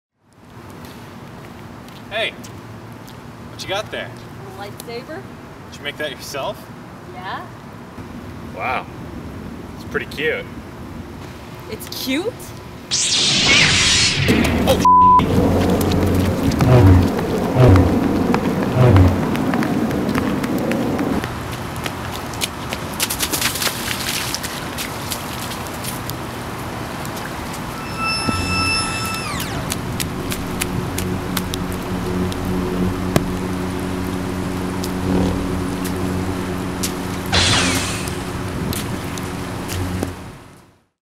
Jenna's Saber
I decided that making a light saber video is just another one of those things you have to do before you graduate from a digital arts degree program.